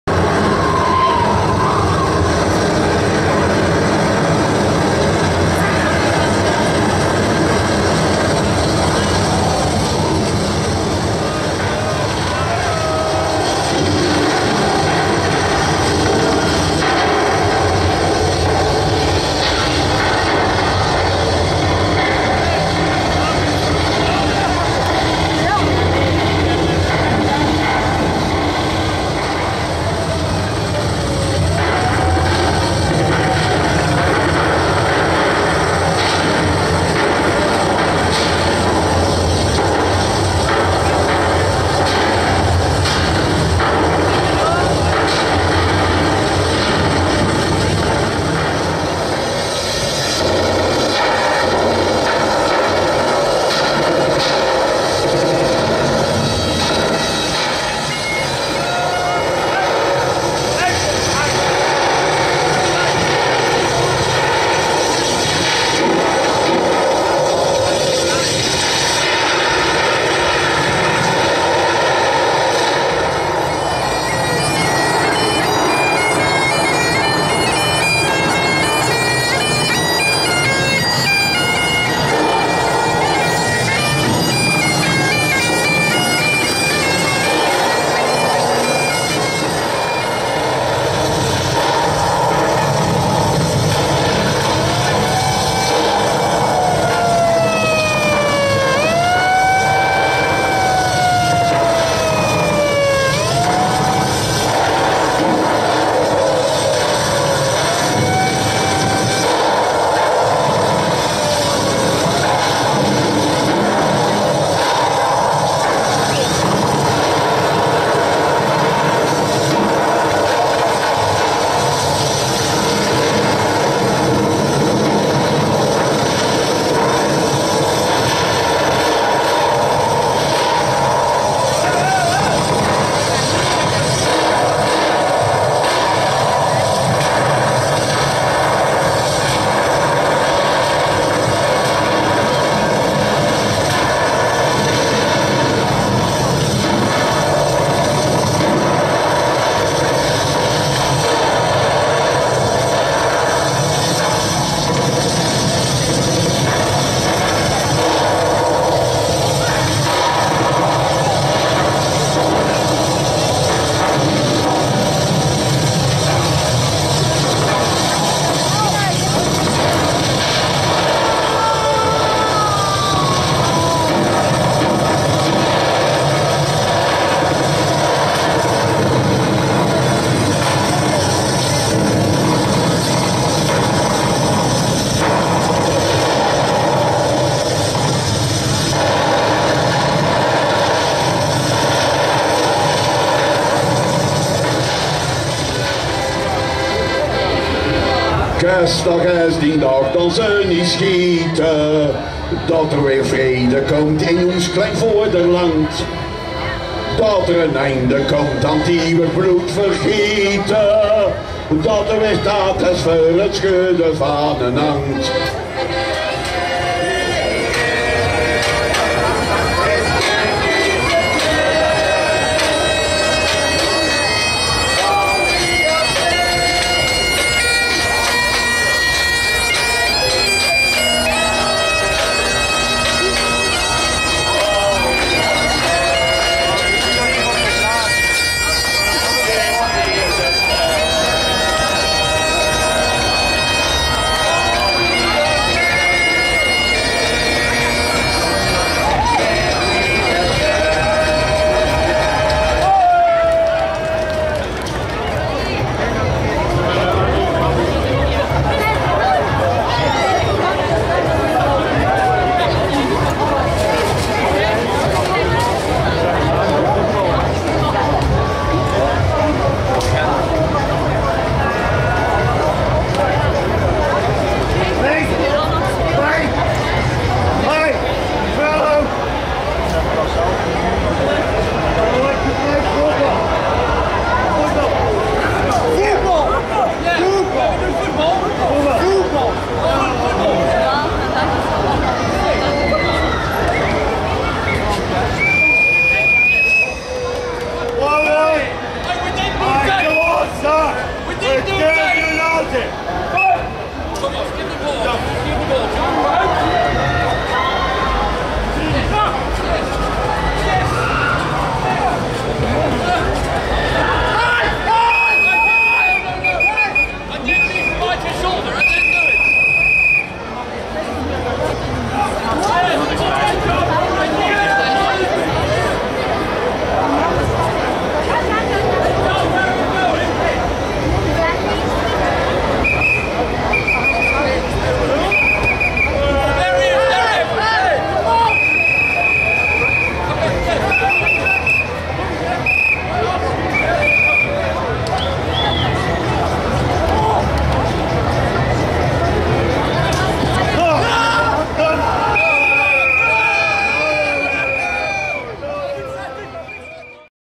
't Spelleke van Drei Kluite een "Gensche Fiestekoenfeeranse"
Op tekst van Freek Neirynck en Luk De Bruyker speelt 't Spelleke van Drei Kluite een "Gensche Fiestekoenfeeranse" met historische hangijzers en schietgeweren, gericht op de Gentse poletiek en met nieuwe Gentse volksliedjes.